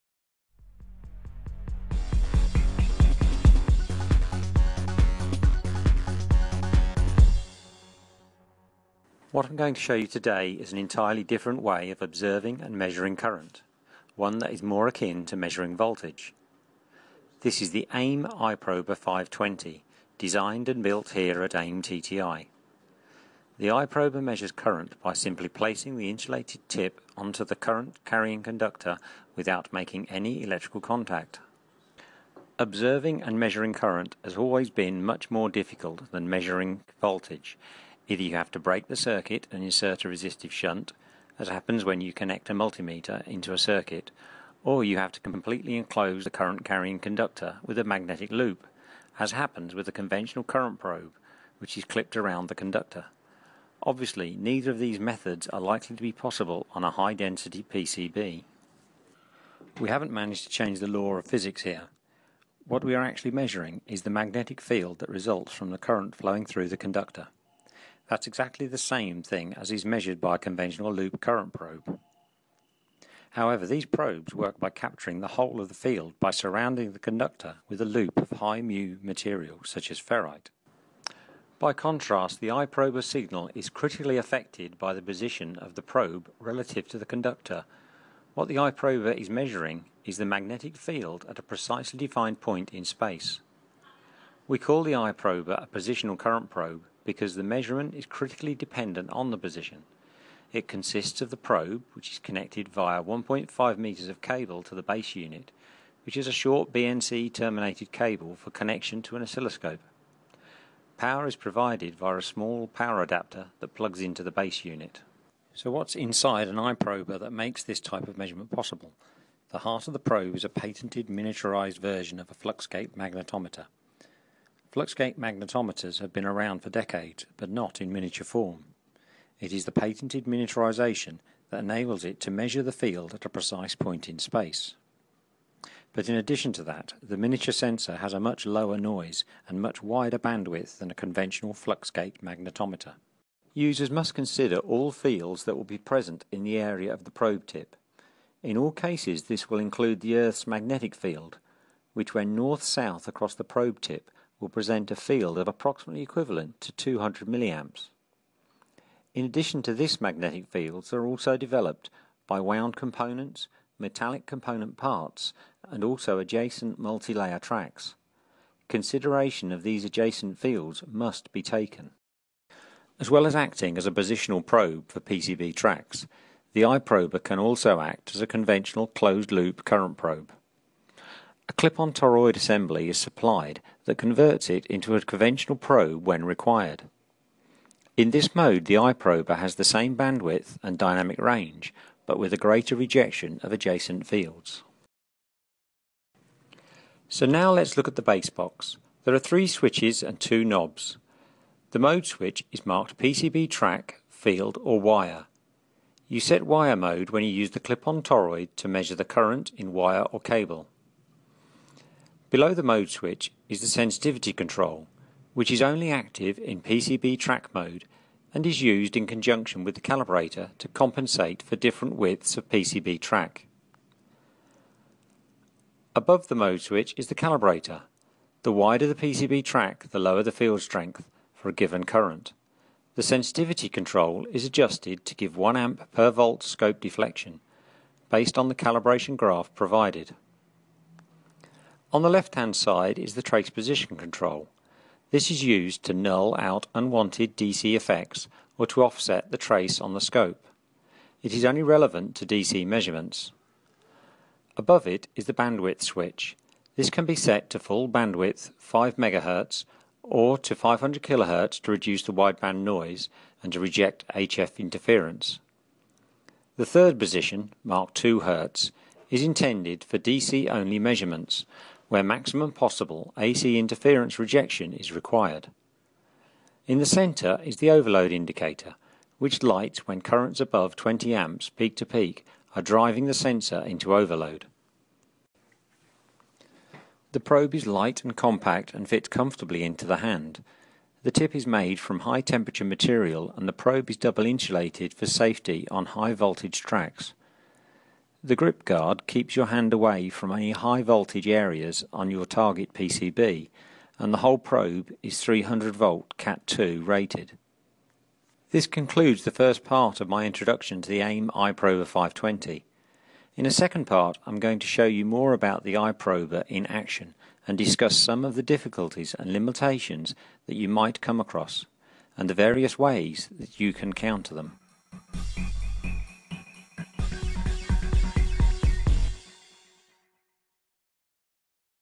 Aim-TTi I-prober 520 Positional Current Probe
The Aim-TTi I-prober 520 is a unique current measurement device. Measurement of current normally requires it to be passed through a closed magnetic loop, typically using a split clamp device. Whereas this is suitable for individual wires, it is of no use for measuring current in PCB tracks. The I-prober 520 uses positional H-field measurement. By placing the insulated tip of the probe onto a PCB track, the current flowing in the track can be observed and measured. The I-prober 520 is supplied with a clip-on toroid assembly which converts it into a closed magnetic circuit probe for measuring current within a wire.